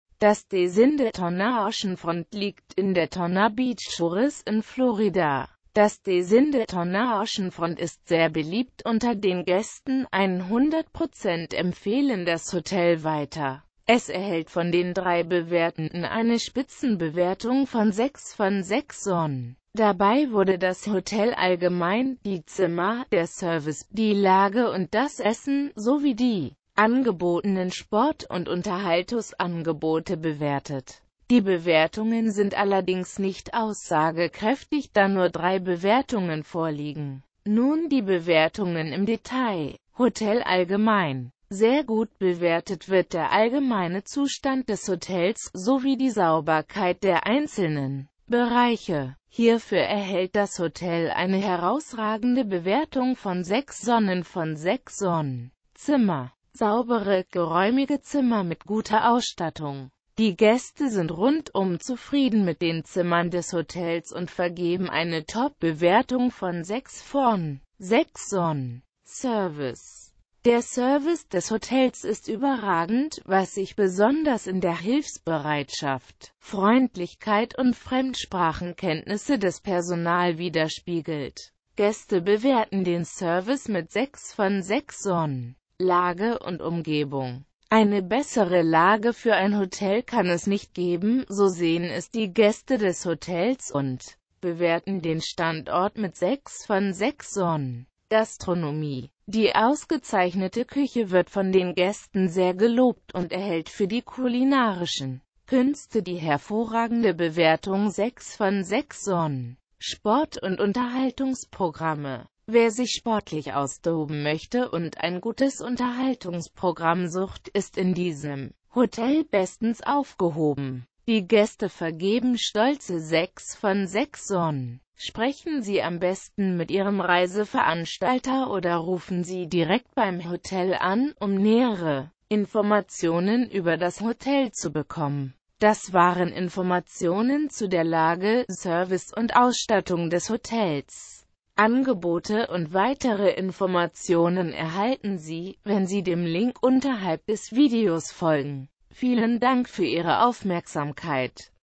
Days Inn Daytona Oceanfront in Daytona Beach Shores (Florida - USA) Bewertung und Bilder
Das Days Inn Daytona Oceanfront liegt in der Stadt Daytona Beach Shores welche zu USA (Nordamerika) gehoert.

 Das Days Inn Daytona Oceanfront ist sehr beliebt unter den Gaesten, 100% empfehlen das Hotel weiter. Zudem erhaelt das Hotel 6 von 6 Sonnen. Die Hotelbewertungen sind allerdings nicht aussagekraeftig, da nur 3 Bewertungen vorliegen. Sprechen Sie am besten mit Ihrem Reiseveranstalter oder rufen Sie direkt beim Hotel an, um mehr Informationen ueber das Hotel zu bekommen.
Hotel allgemein:
 Sehr gut bewertet wird der allgemeine Zustand des Hotels, sowie die Sauberkeit der einzelnen Bereiche. Hierfuer erhaelt das Hotel eine herausragende Bewertung von 6 Sonnen von 6 Sonnen.
Zimmer:
 Saubere, geraeumige Zimmer mit guter Ausstattung. Die Gaeste sind rundum zufrieden mit den Zimmern des Hotels und vergeben eine top Bewertung von 6 von 6 Sonnen.
Service:
 Der Service des Hotels ist ueberragend, was sich besonders in der Hilfsbereitschaft, Freundlichkeit und Fremdsprachenkenntnisse des Personal wiederspiegelt. Gaeste bewerten den Service mit 6 von 6 Sonnen.
Lage und Umgebung:
 Eine bessere Lage fuer ein Hotel kann es nicht geben, so sehen es die Gaeste des Hotels und bewerten den Standort mit 6 von 6 Sonnen.
Gastronomie:
 Die ausgezeichnete Kueche wird von den Gaesten sehr gelobt und erhaelt fuer die kulinarischen Kuenste die hervorragende Bewertung 6 von 6 Sonnen.
Sport und Unterhaltungsprogramme:
 Wer sich sportlich austoben moechte und ein gutes Unterhaltungsprogramm sucht, ist in diesem Hotel bestens aufgehoben. Die Gaeste vergeben stolze 6 von 6 Sonnen.

Informationen zur Verfuegbarkeit von Transfer zum Hotel, WLAN in den Zimmern des Days Inn Daytona Oceanfront, sowie Details zum Animations-Progamm, Entfernung zum Strand oder zu Diskotheken erhalten Sie bei Ihren Reiseveranstalter oder auf der Webseite des Hotels. Neben den hier verwendeten Bewertungen, lohnt sich ein Blick auf die ggf. vorhandene Days Inn Daytona Oceanfront Facebook-Seite.

Quellenhinweis:
Die verwendeten Informationen stammen von HolidayCheck.de. Sie wurden am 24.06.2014 abgerufen. In diesem Video wurden Bilder aus Daytona Beach Shores oder allgemeine Bilder aus USA verwendet. Die gezeigten Orte und Landschaften dienen als Beispiel und muessen nicht unbedingt in der Naehe des Hotels liegen.